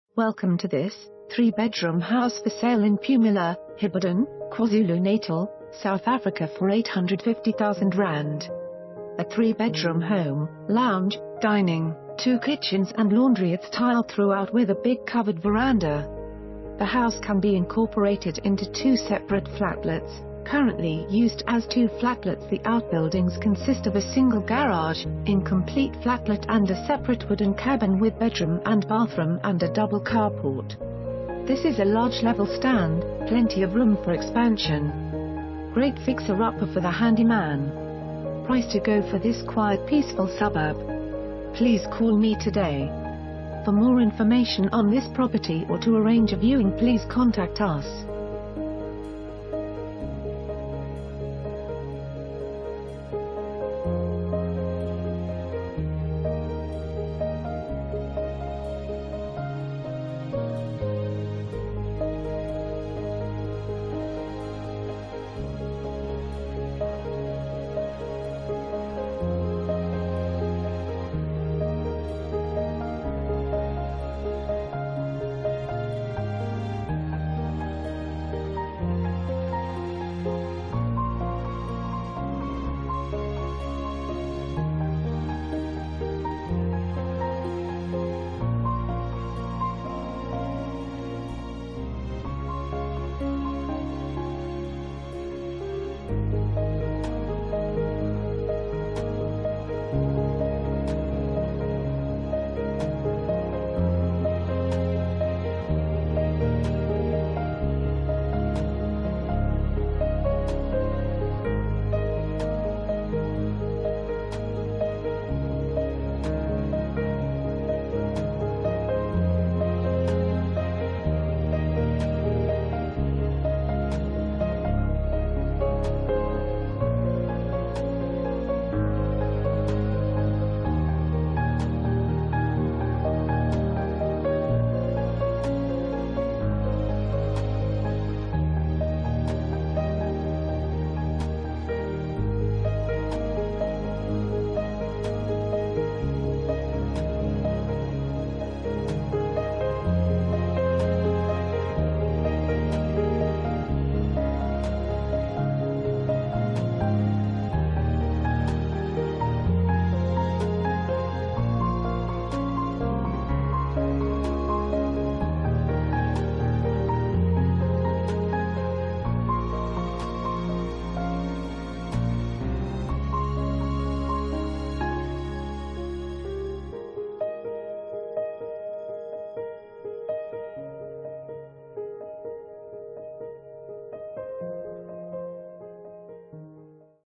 3 Bedroom House For Sale in Pumula, Hibberdene, KwaZulu Natal, South Africa for ZAR 850,000
A 3 bedroom home, lounge, dining, two kitchens and laundry it's tiled throughout with a big covered veranda. The house can be incorporated into two separate flatlets,( currently used as two flatlets) The outbuildings consist of a single garage, incomplete flatlet and a separate wooden cabin with bedroom and bathroom and a dou...